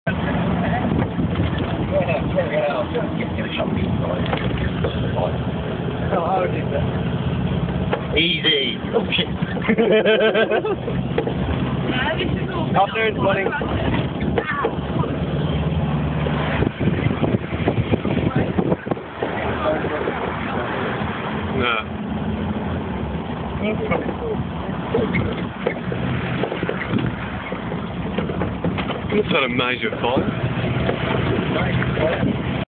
Jeep Safari, Bodrum 1
Driving around Bodrum, Turkey. 2007